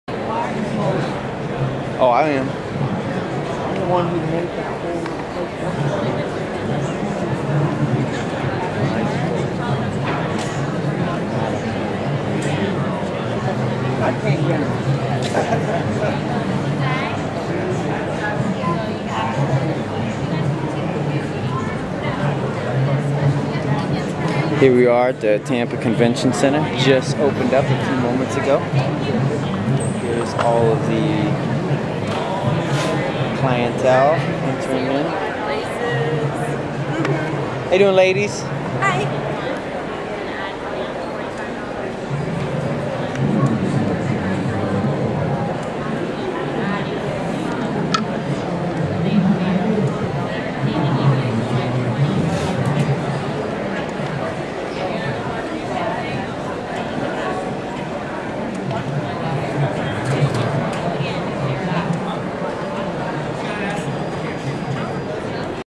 Cloud Report: Gasparilla Expo Opens, Hundreds Stream In
The first major US-based Expo for the On-Running shoes occurred in Tampa, FL at the 2011 Publix Super Markets Gasparilla Distance Classic Race.